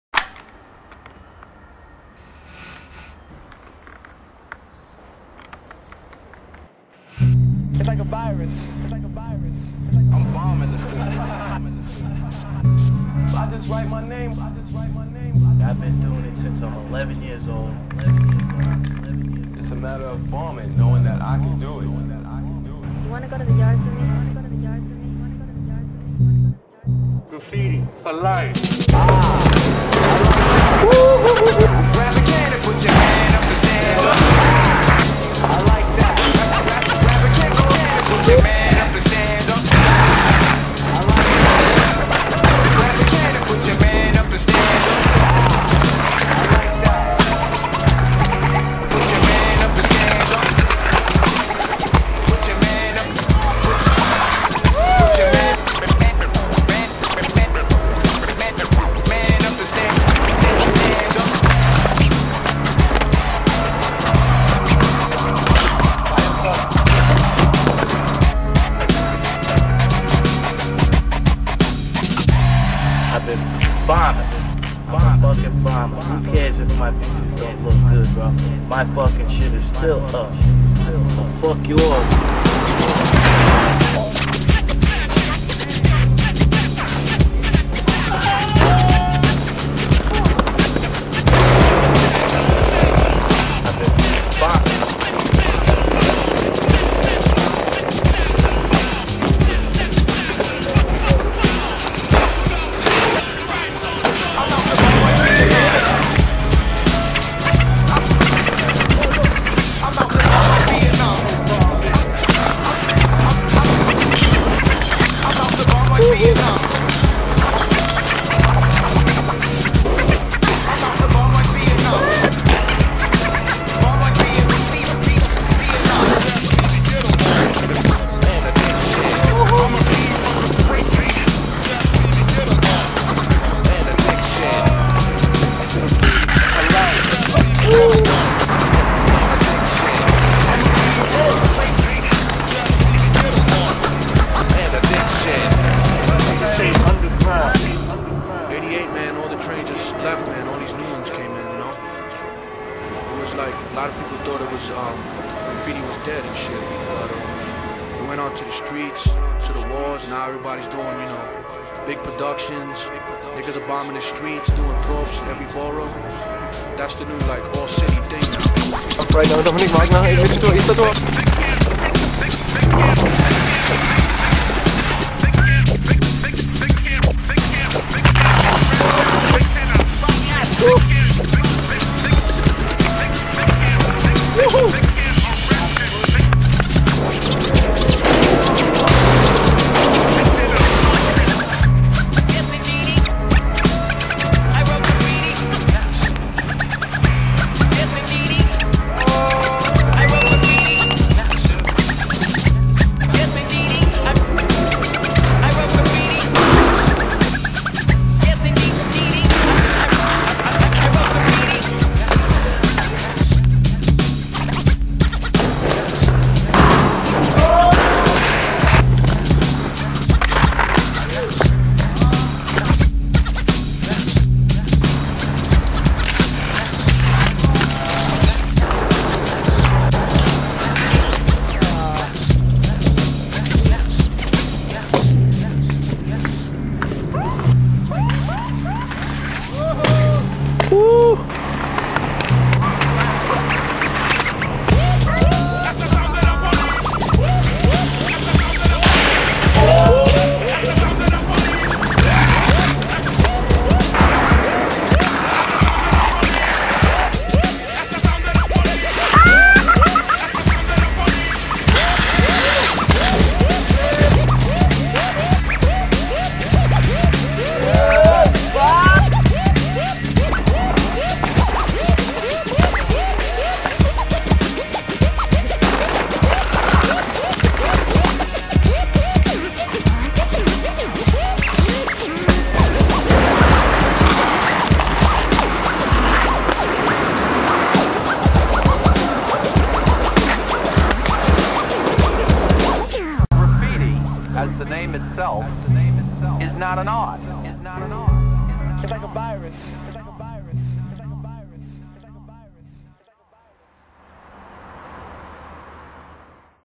Skating and graffiti
edit filmed in spring-summer 2007 in romania, hungary, germany, croatia including skaters from everywhere.